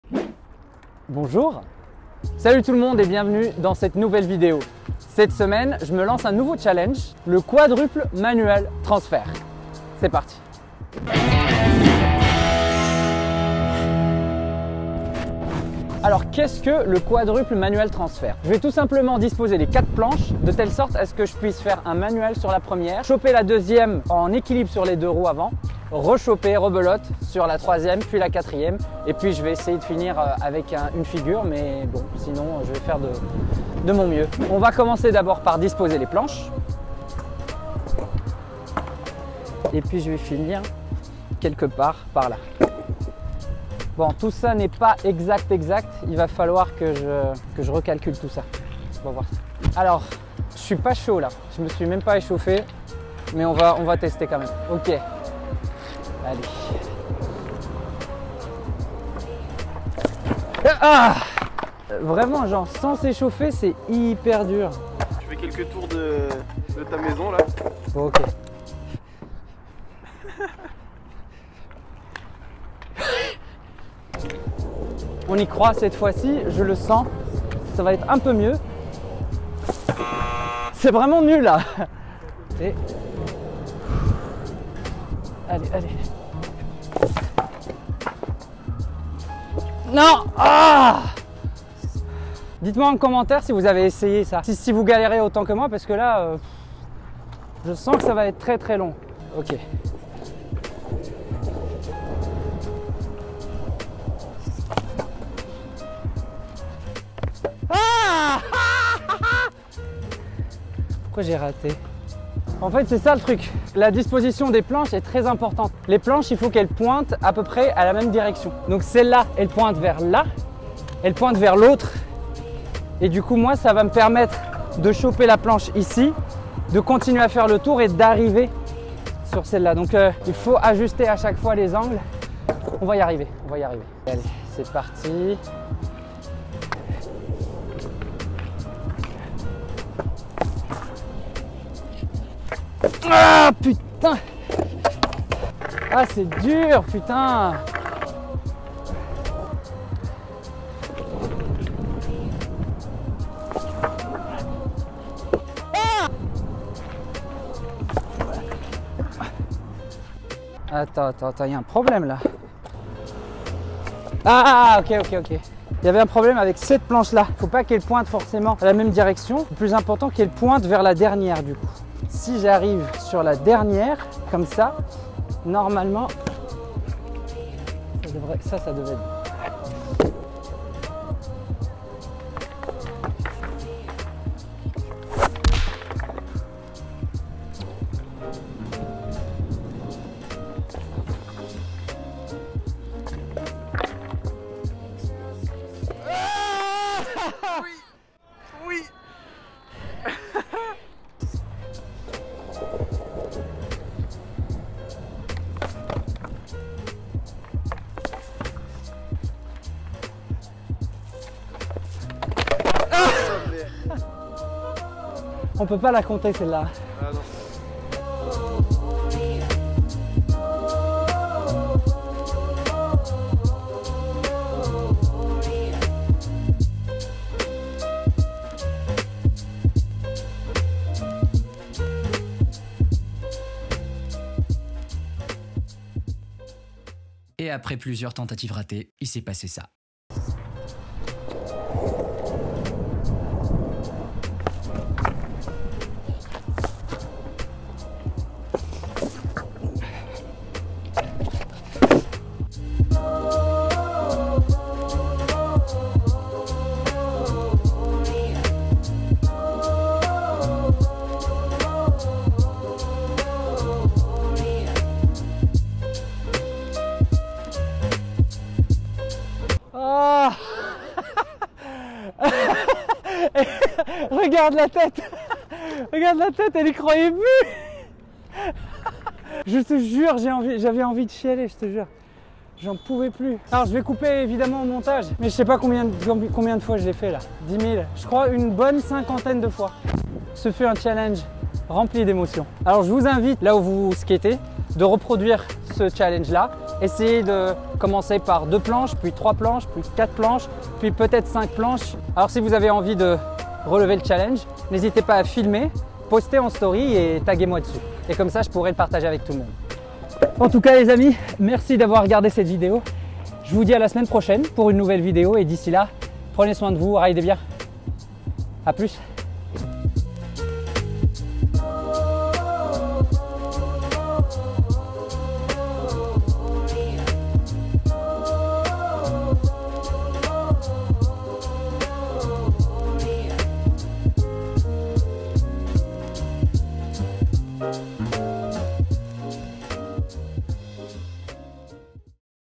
LONGBOARD CHALLENGE #2 : Quadruple Manual transfert
Pour ce 2ème challenge je vais essayer de faire un quadruple manual transfert ! 😳
Plus d’infos sur les planches que je ride

Un grand Merci à Tifenn et Simon pour leur aide et leur patience ❤️